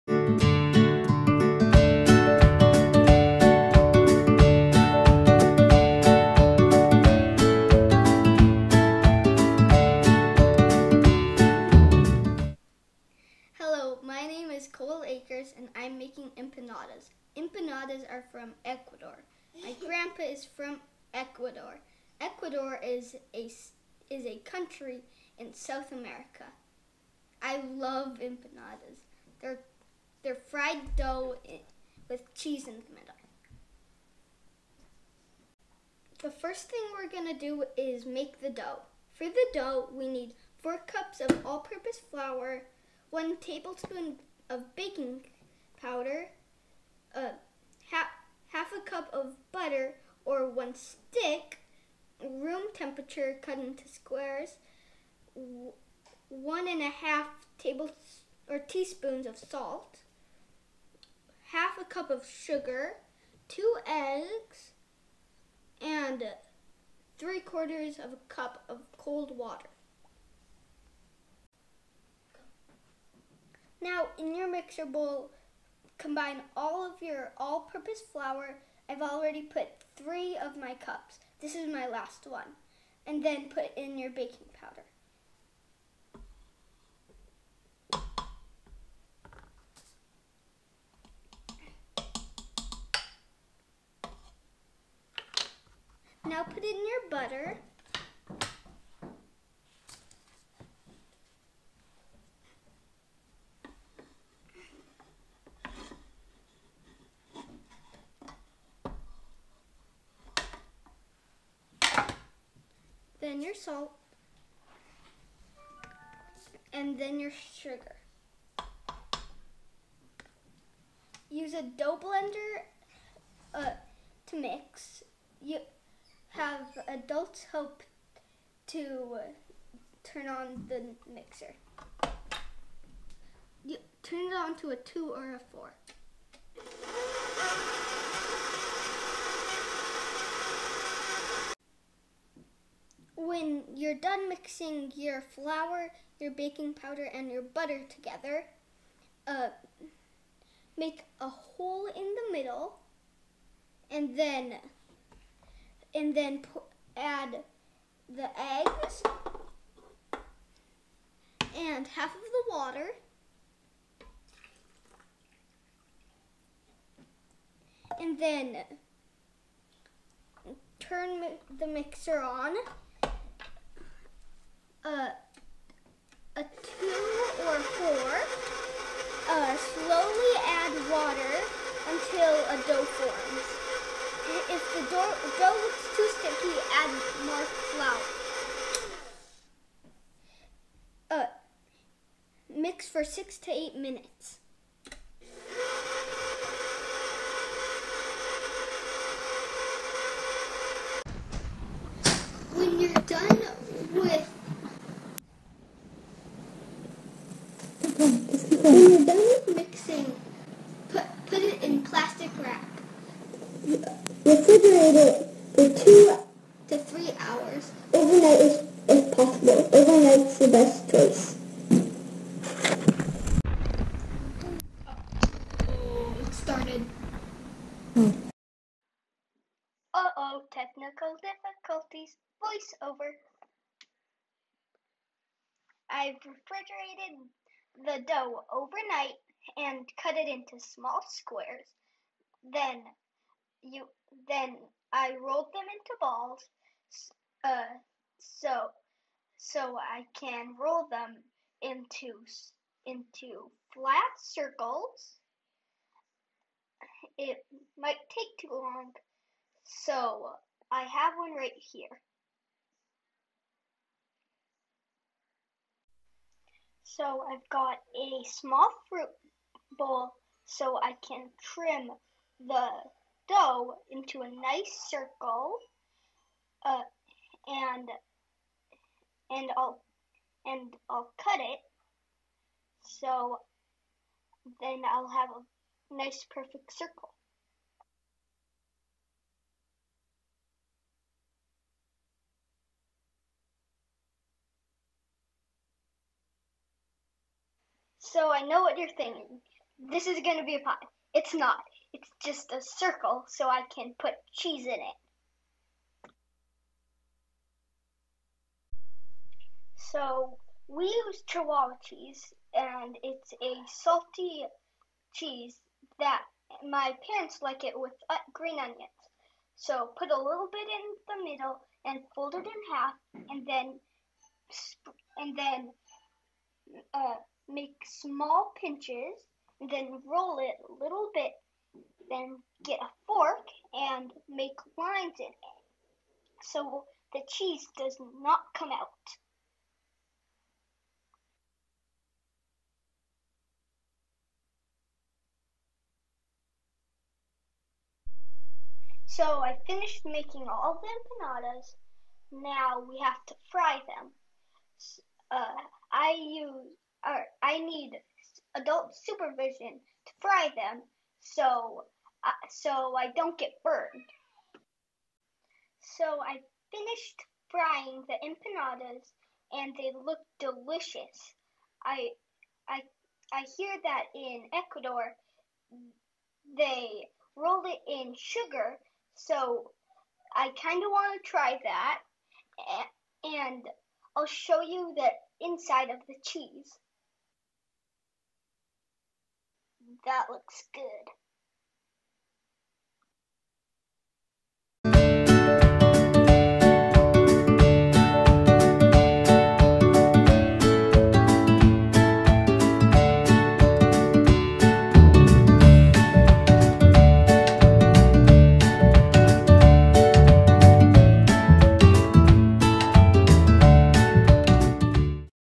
Cooking with Cole- Ecuadorian Empanadas
Akers Family Kitchen teaches you how to make Cheesy Ecuadorian Empanadas